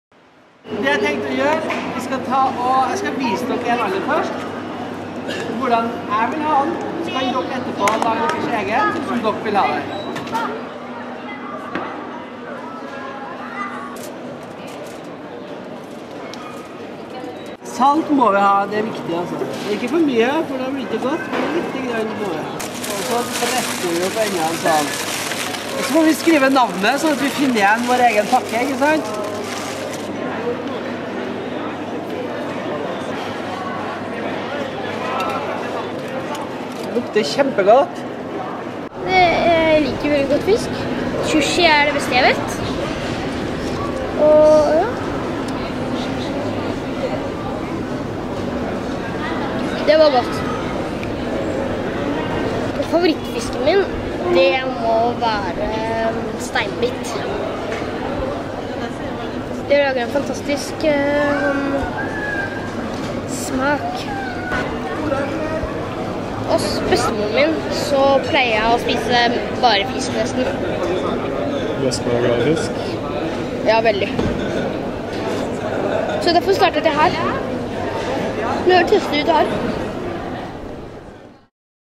Matkurs for barn
God Fisk og Kulinarisk Akademi arrangerer matkurs for barn på Mathallen i Oslo.